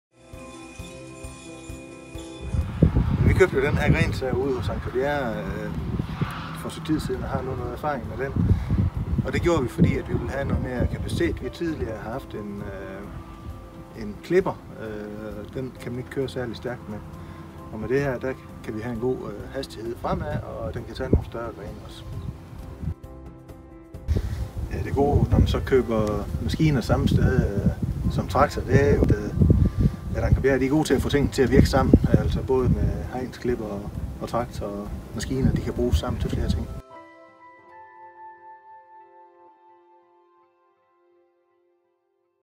Anker Bjerre A/S - Greentec greensav
Hør landmand, Morten Bjerre, fortælle om grensaven fra Greentec - og om grunden til at han har valgt at købe grensav, traktor og andre maskiner hos Anker Bjerre A/S.